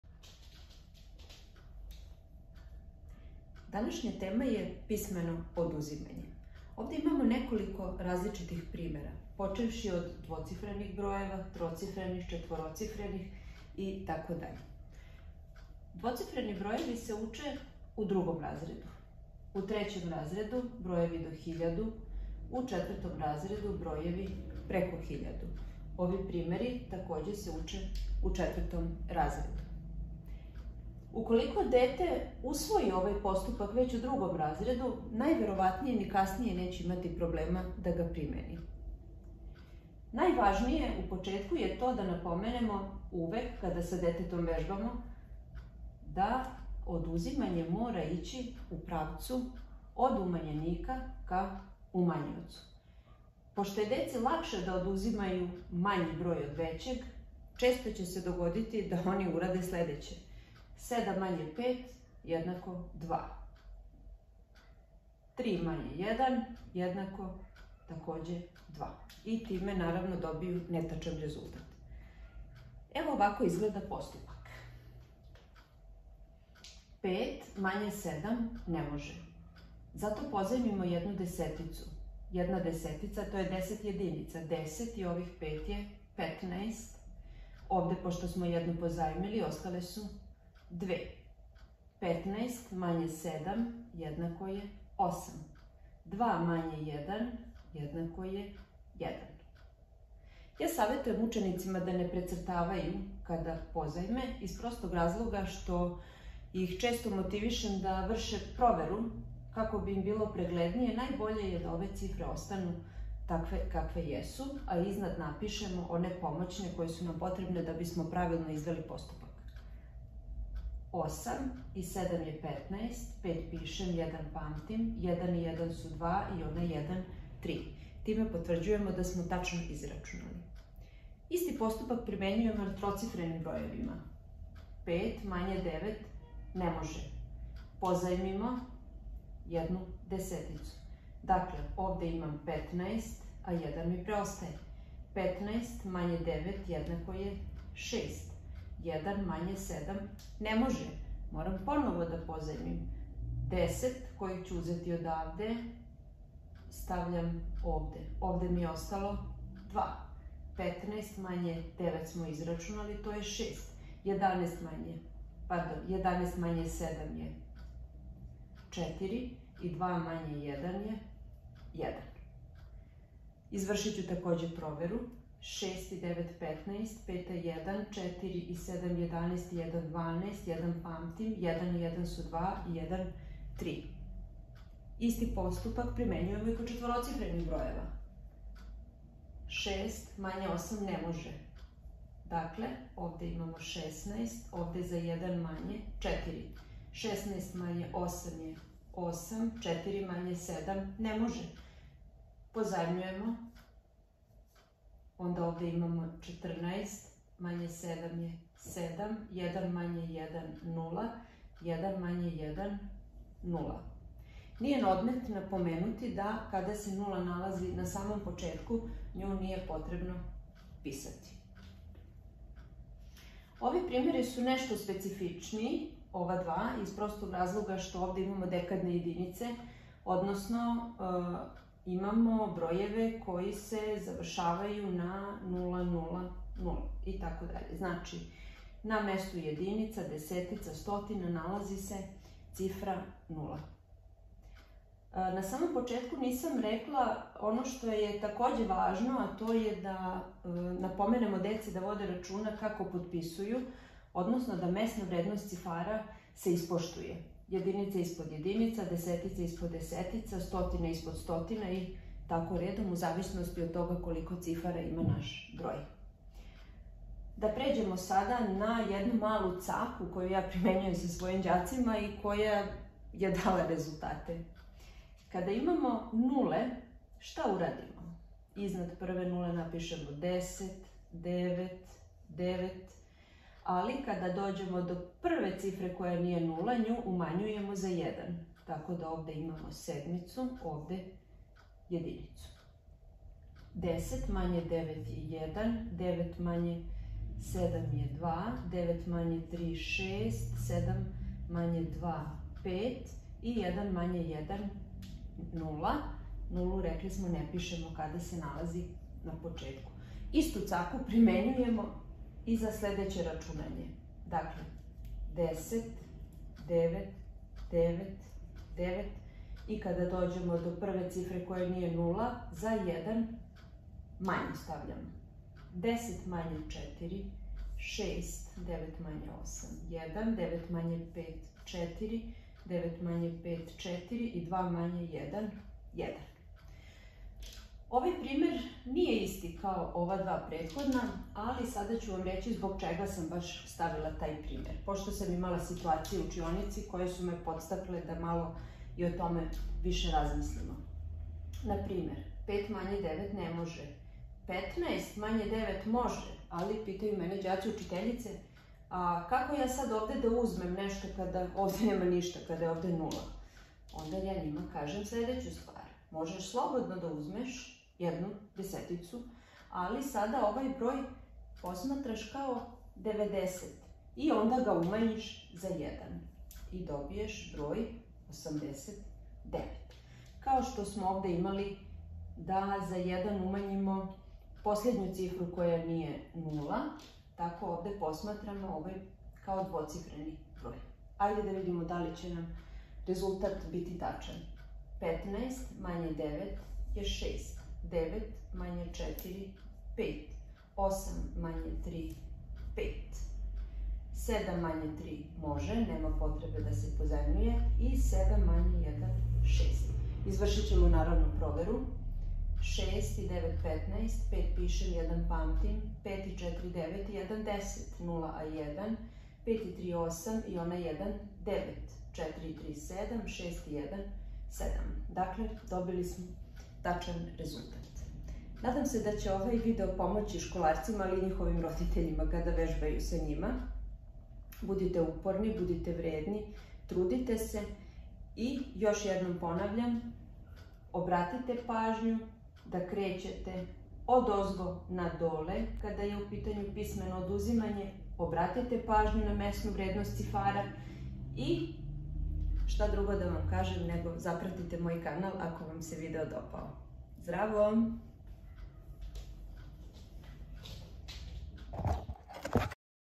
Pismeno oduzimanje brojeva
Postupak pismenog oduzimanja brojeva sa prelazom preko desetice-od dvocifrenih do visecifrenih brojeva.